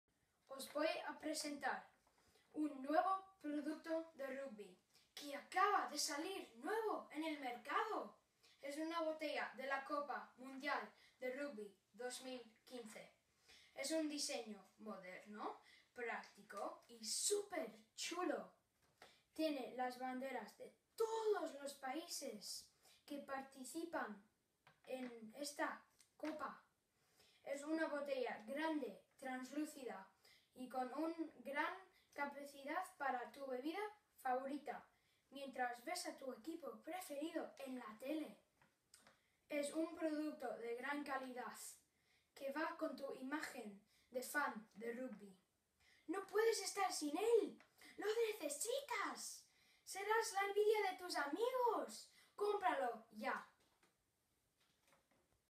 Joint 3rd place Year 7-9 category - Daniel Ramos-Brown - Grammar School at Leeds 1 file
Could you design a sports kit or a water bottle for the Rugby World Cup using a foreign language element? Do you have the IT skills to make a video advert or business pitch to accompany your design?

These are the questions that were put to Year 7-11 pupils across in Yorkshire and The Humber on the unveiling of a new and innovative language competition. Launched on the European Day of Languages and to tie-in with the 2015 Rugby World Cup, a new competition was born to link together languages and design in a way never previously undertaken.

Pupils entering the competition in Years 7-9 were asked to submit a video advert of no more than 2 minutes with their sports kit or water bottle design. Those in Years 10-11 were required to submit a business pitch in the target language, demonstrating the benefits of their design. All languages were accepted as long as they were part of the school curriculum.

7 schools from across the region submitted multiple entries with colourful, creative and fun designs, along with excellent language elements in Spanish, French, German and Urdu. The video adverts and pitches were lively and entertaining and the pupils really sold their products. It was fantastic to see so many pupils of all abilities getting involved and producing high quality work. The competition definitely captured the imagination of boys as well as girls, partly due to the design and sport angle of the competition.